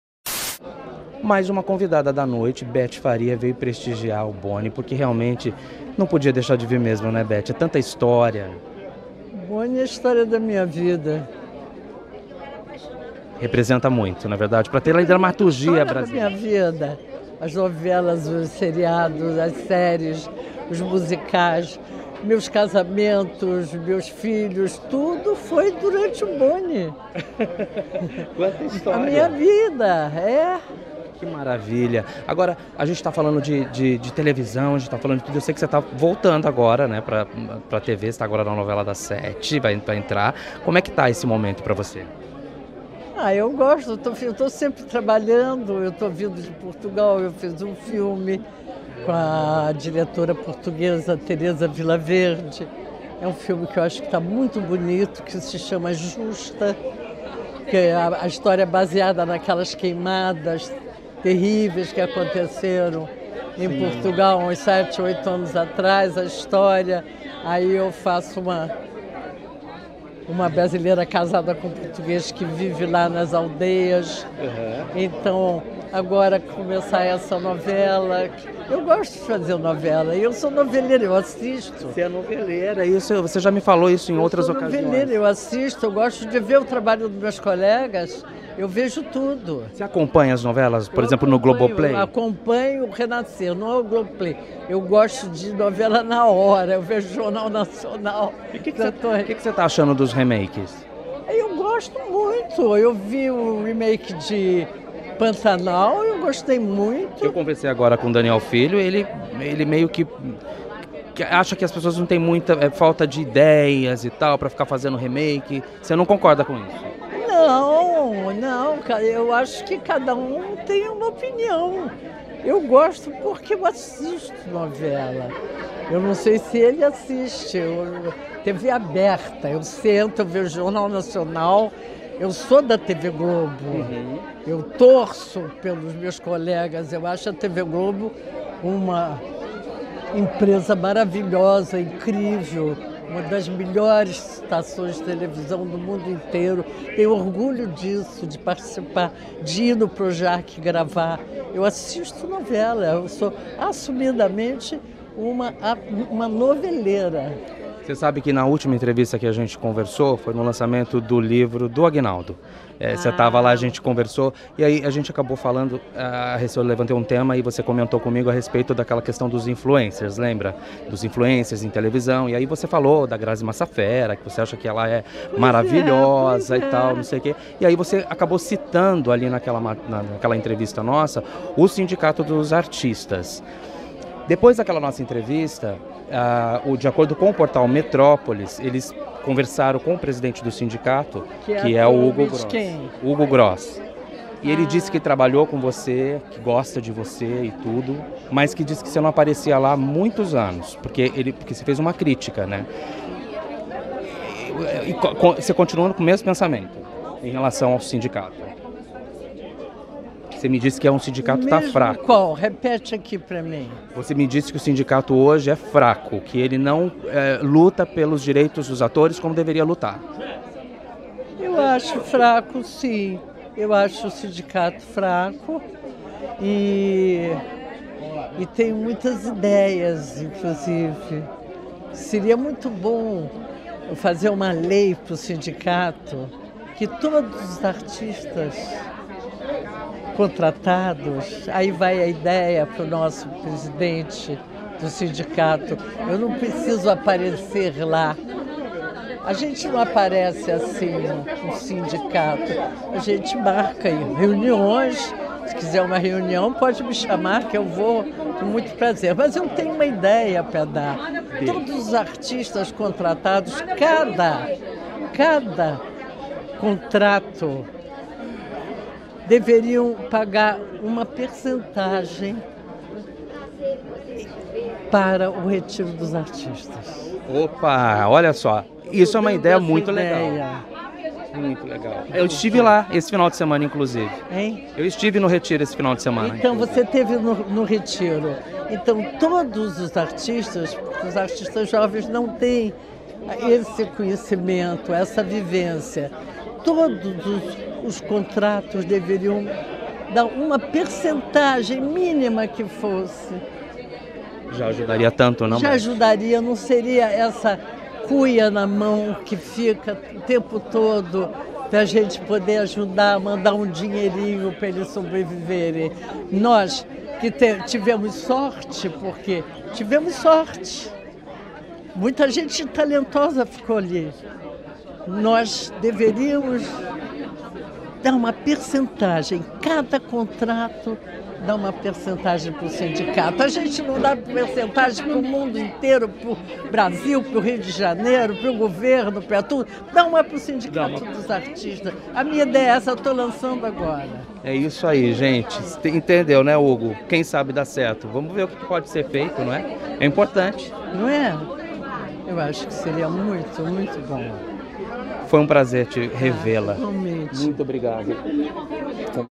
BETTY FARIA rebate presidente do Sindicato dos Artistas e dá sugestão: "muitas ideias"
A atriz Betty Faria conversou com o jornalista Marcos Bulques durante o lançamento do livro "O lado B do Boni",  de José Bonifácio de Olivera Sobrinho, o Boni, que aconteceu no Rio de Janeiro. Na ocasião, ela rebateu declarações do presidente do Sindicato dos Artistas, Hugo Gross, sobre não ter participação ativa na entidade. Veja!